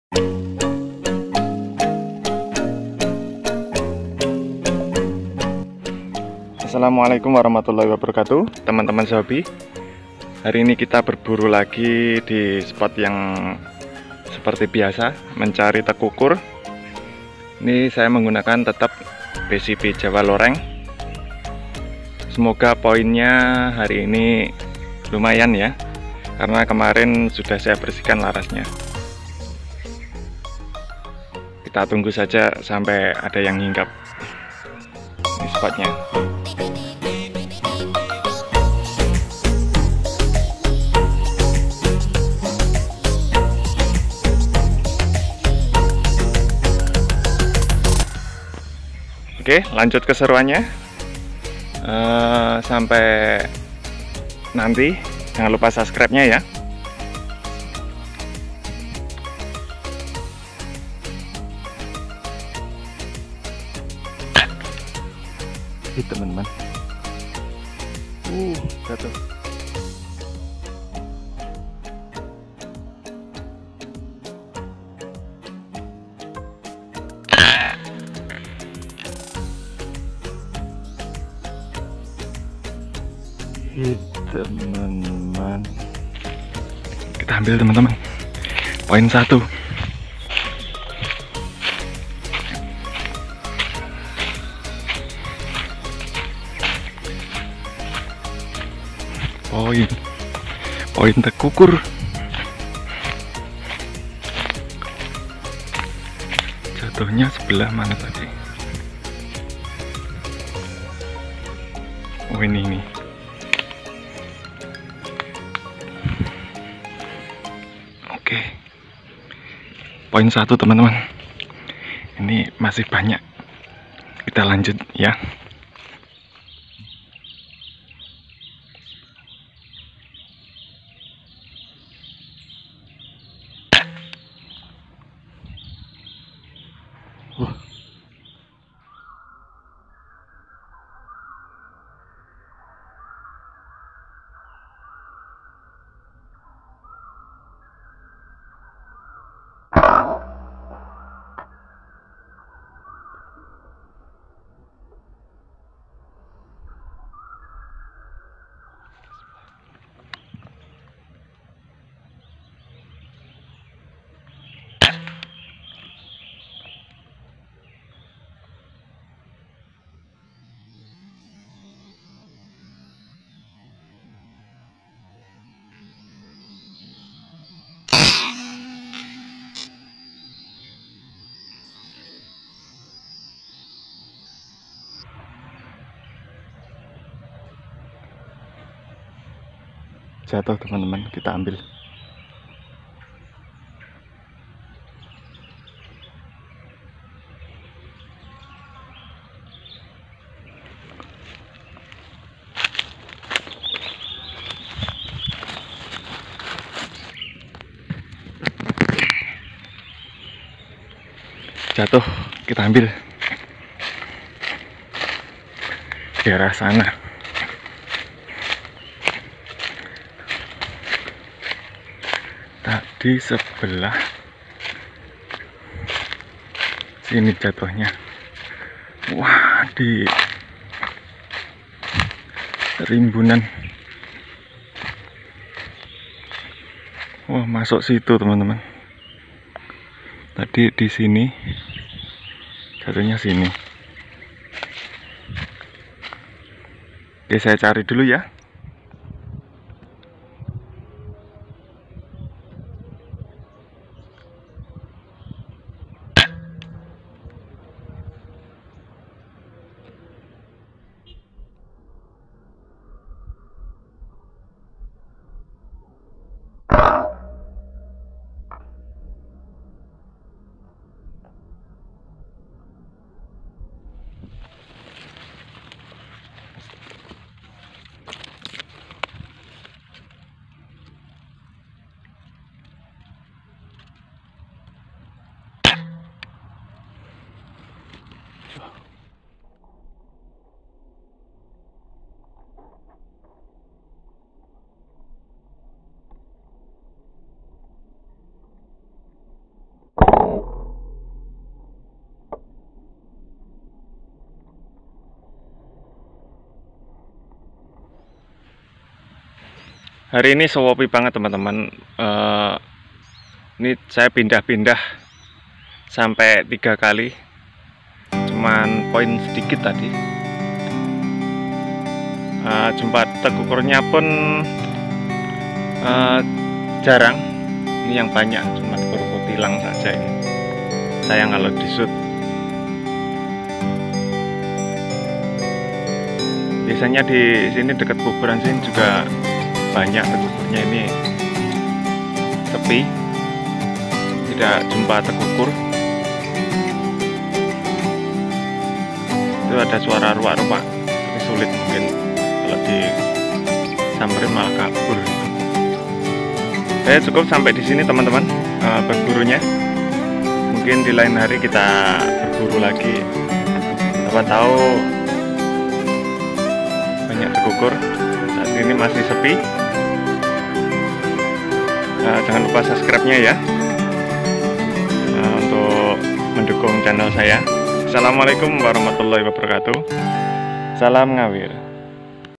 Musim tekukur waktunya poin banyak
Masih sepi teman-teman pagi ini tapi Alhamdulillah masih di beri poin